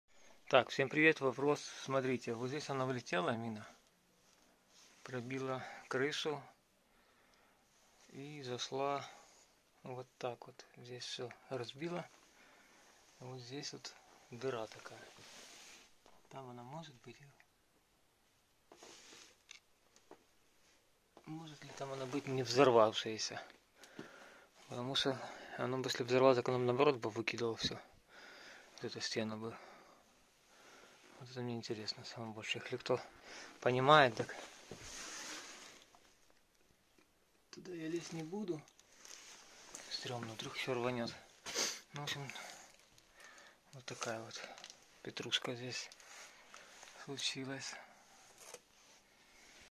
В мой дом попали с миномета😭 | Возможно ли такое что там мина целая?
Вопрос к тем кто понимает 🤔